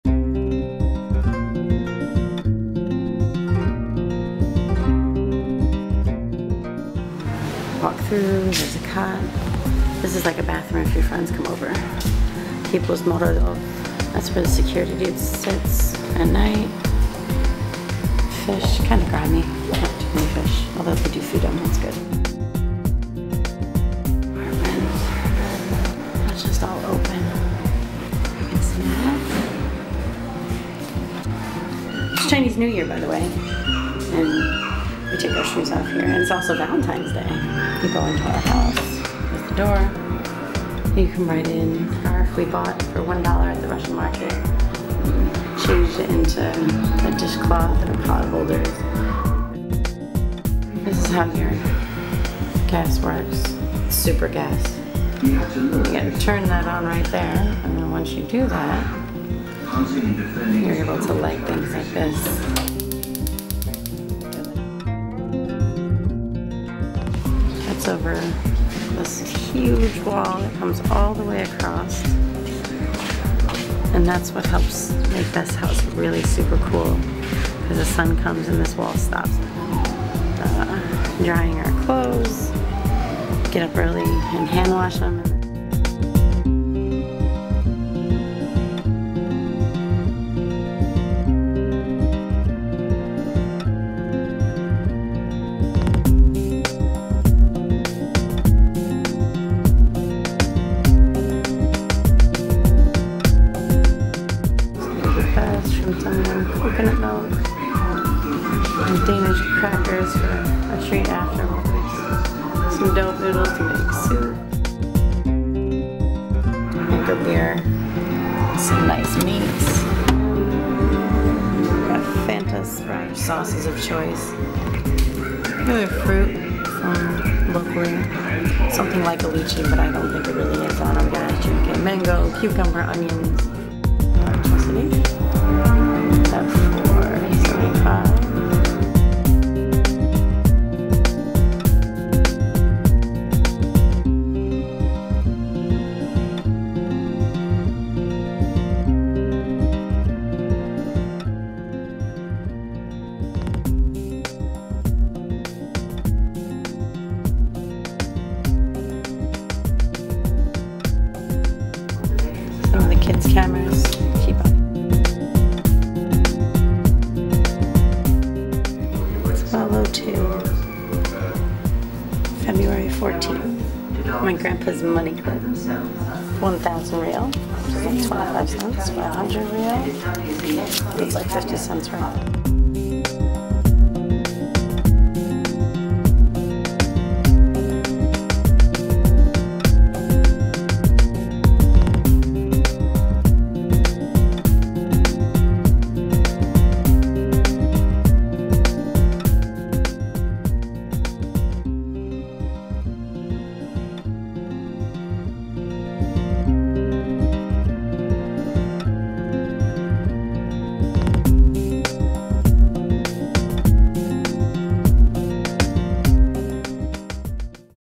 HOME in Phnom Penh, Cambodia 2.14.2010
Take a tour through our flat in Cambodia. While we are fighting for futures this will act as our headquarters.